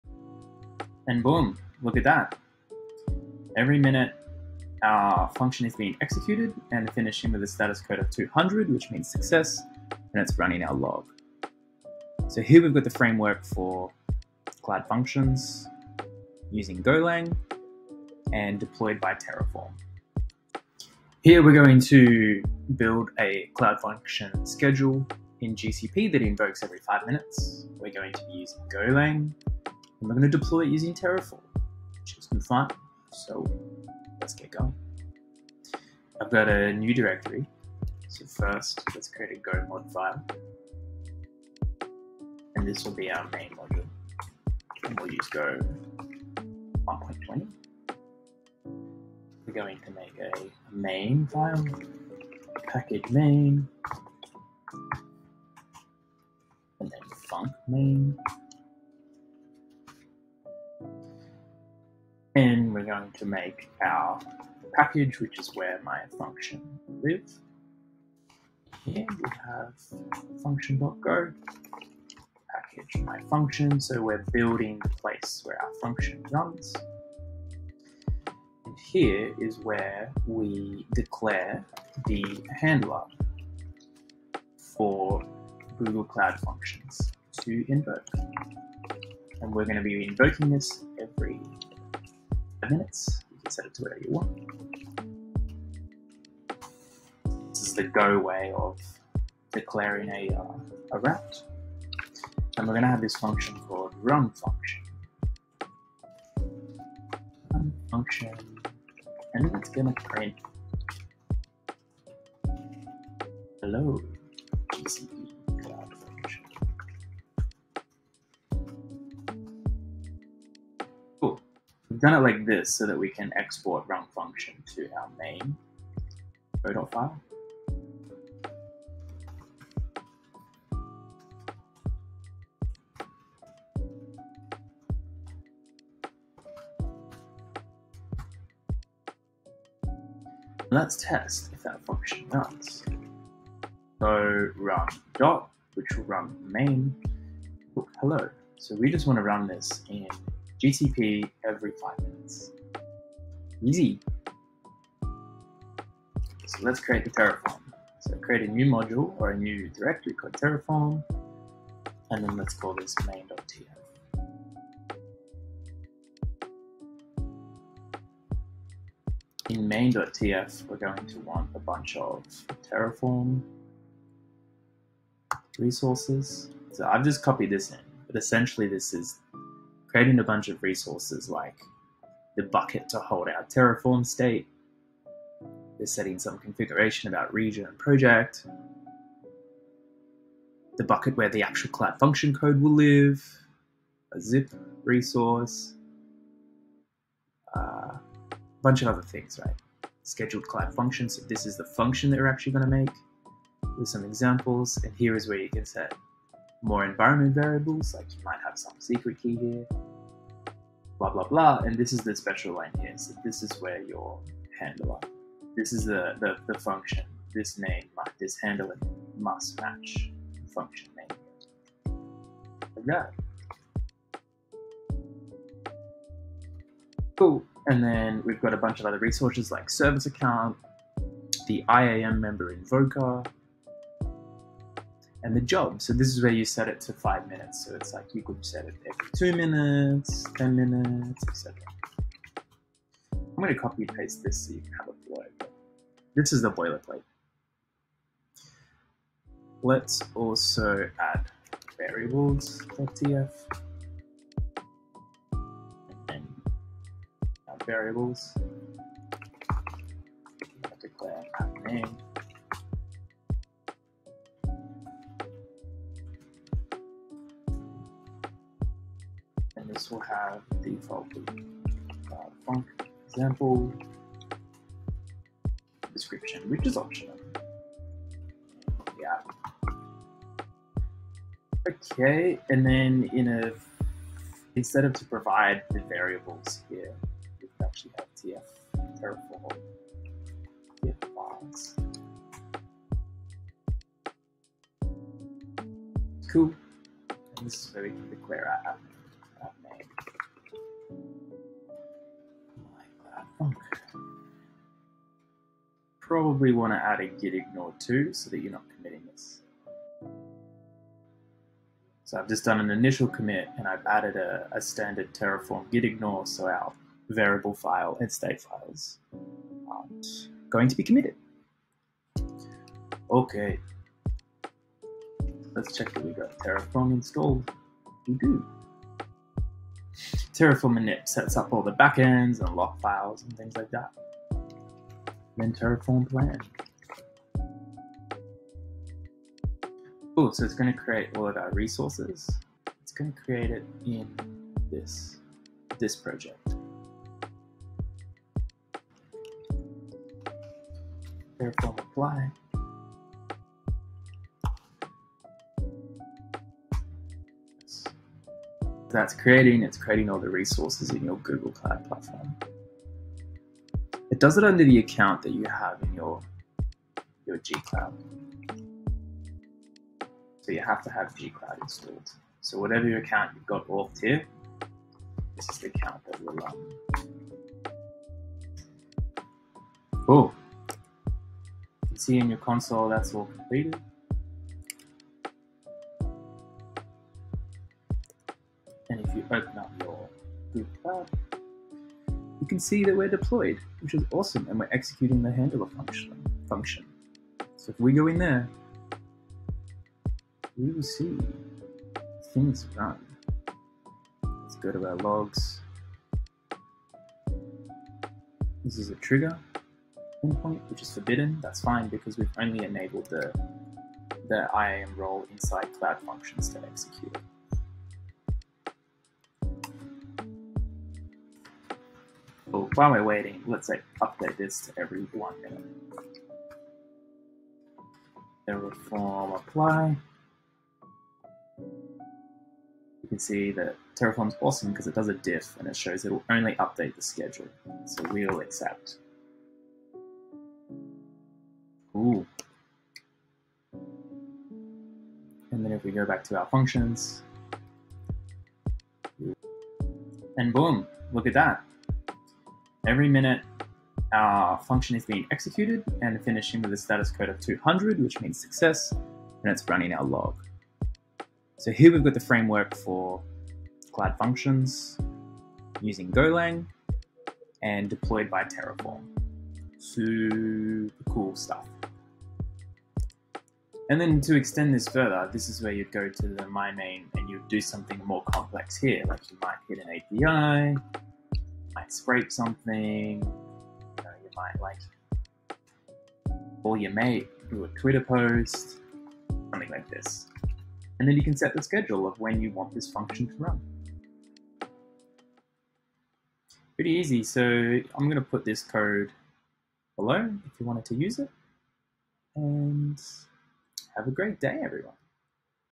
Run golang serverless with terraform - easier than you think
Ever wanted your serverless function to run on a schedule? Dah, we all do 😋
Here's a quick setup of Golang + Terraform + Google Cloud Platform makes that soo easy.

- Setup go lang handlers
- Initialise terraform
- Deploy to GCP Google Cloud Platform

shredder head out